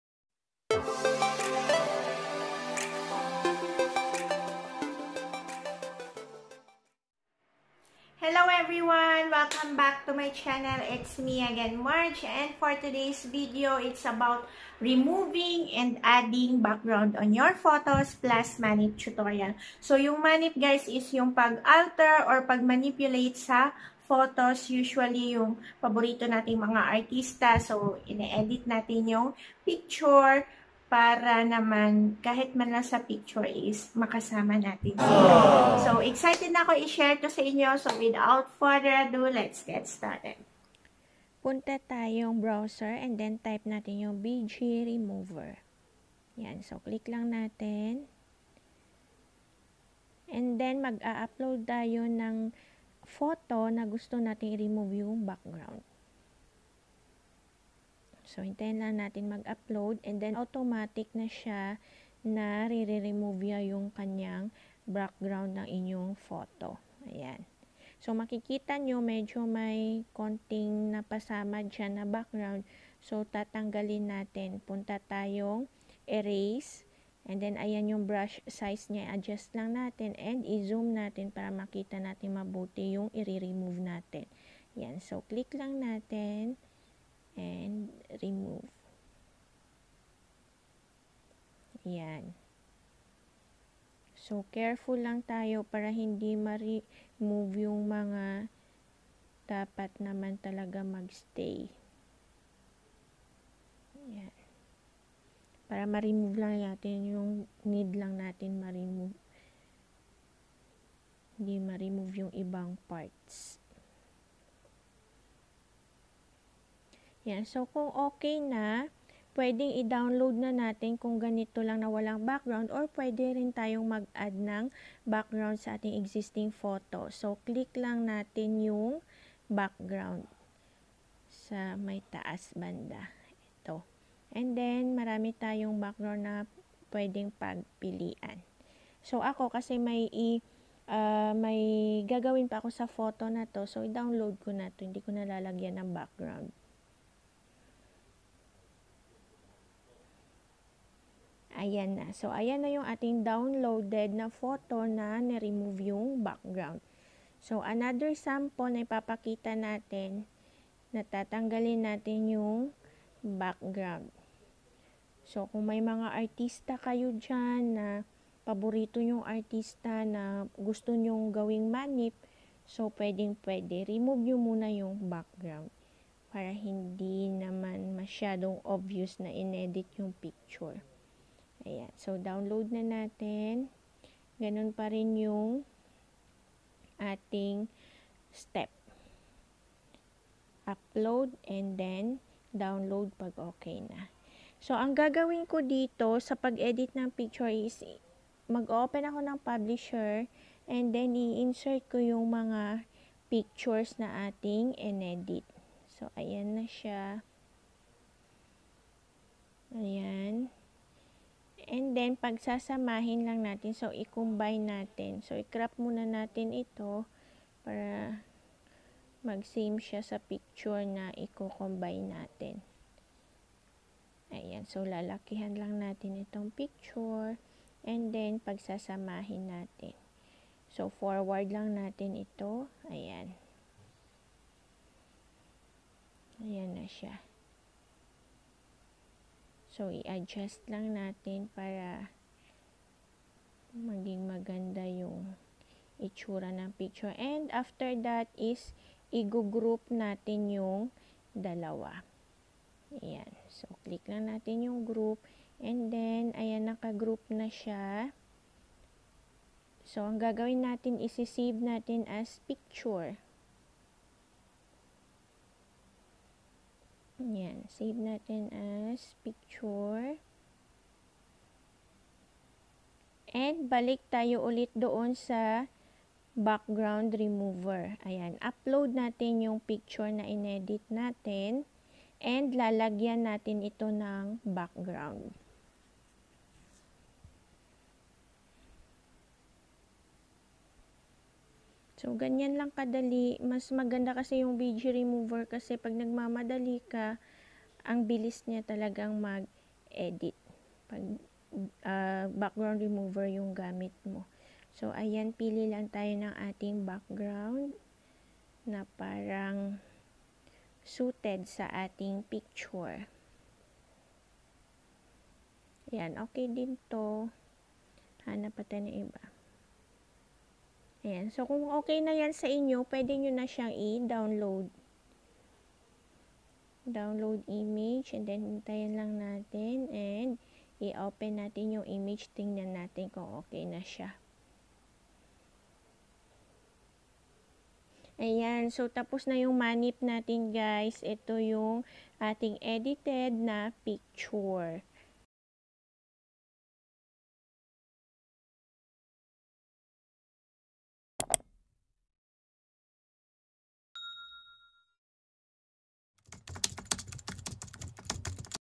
Removing/Adding Background of Photos/ Manip Tutorial
This video will show you how to remove and add background of your photos using background remover.